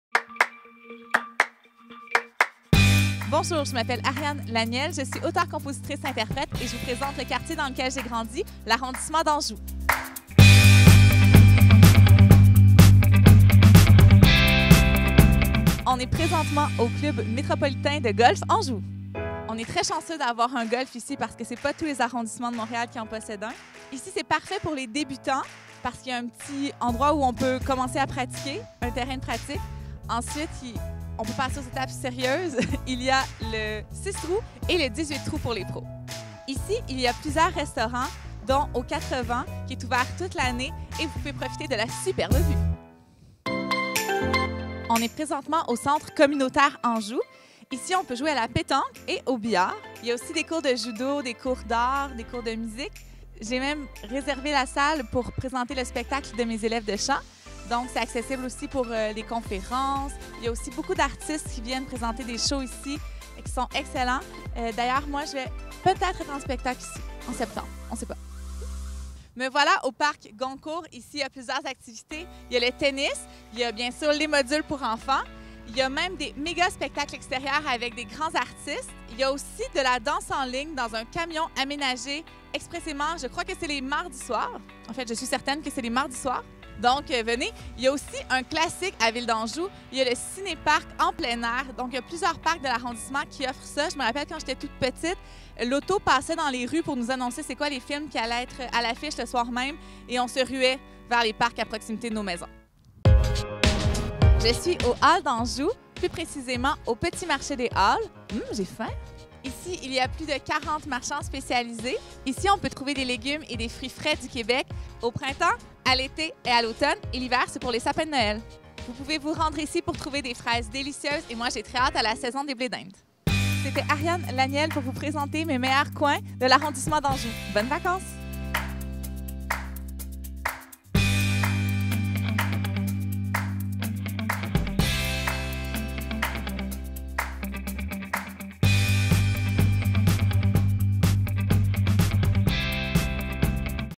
Le quartier d'Ariane Laniel: Anjou
Découvrez l'arrondissement d'Anjou avec l'auteure-compositrice-interprète Ariane Laniel.